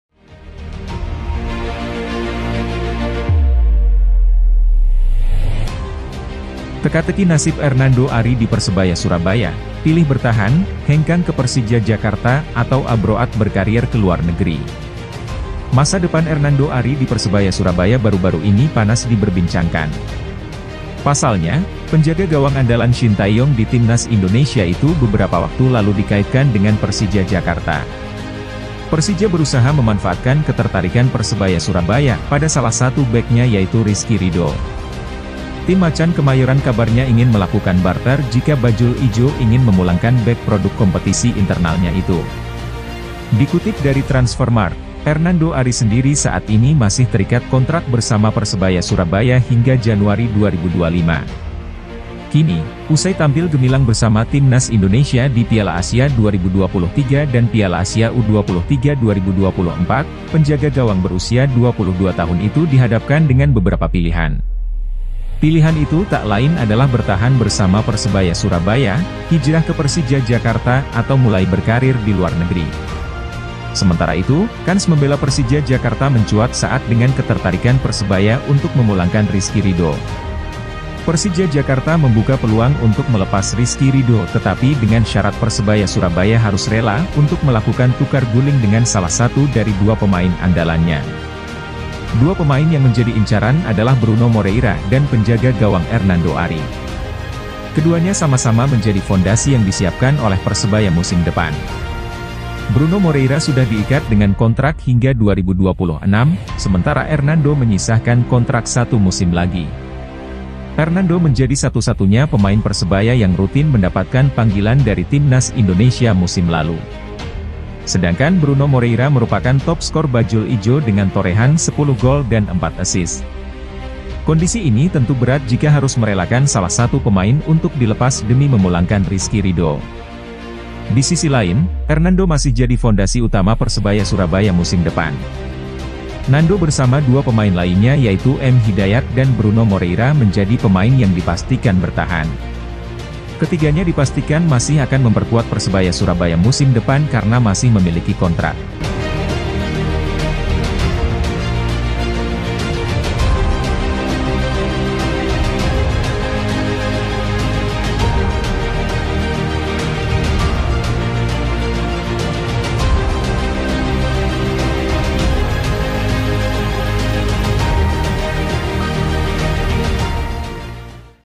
Persebaya Surabaya Siap Cari Pengganti Jika Ernando Ari Hengkang
BERITA PERSEBAYA SURABAYA - Teka-teki nasib Ernando Ari di Persebaya Surabaya, pilih bertahan, hengkang ke Persija Jakarta atau abroad berkarier ke luar negeri. Masa depan Ernando Ari di Persebaya Surabaya baru-baru ini panas diberbincangkan.

Pasalnya, penjaga gawang andalan Shin Tae-yong di Timnas Indonesia itu beberapa waktu lalu dikaitkan dengan Persija Jakarta. Persija berusaha memanfaatkan ketertarikan Persebaya Surabaya pada salah satu beknya yaitu Rizky Ridho.

Tim Macan Kemayoran kabarnya ingin melakukan barter jika Bajul Ijo ingin memulangkan bek produk kompetisi internalnya itu. Dikutip dari Transfermarkt, Ernando Ari sendiri saat ini masih terikat kontrak bersama Persebaya Surabaya hingga Januari 2025.

Kini, usai tampil gemilang bersama Timnas Indonesia di Piala Asia 2023 dan Piala Asia U23 2024, penjaga gawang berusia 22 tahun itu dihadapkan dengan beberapa pilihan. Pilihan itu tak lain adalah bertahan bersama Persebaya Surabaya, hijrah ke Persija Jakarta atau mulai berkarir di luar negeri.

Jangan lupa memberikan komentar di setiap video yang channel ini tayangkan.
🙏Terimakasih🙏